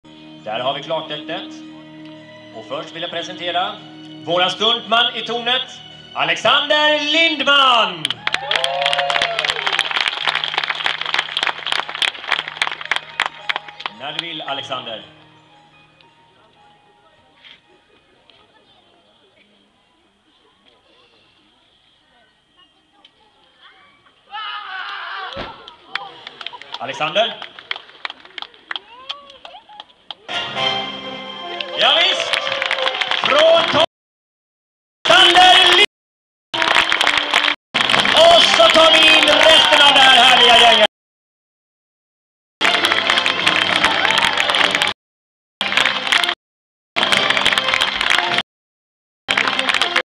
Stunt Alexander Lindman at High Chaparral 2009
Stuntman Alexander Lindman faller från tornet på High Chaparral öppningshelgen 2009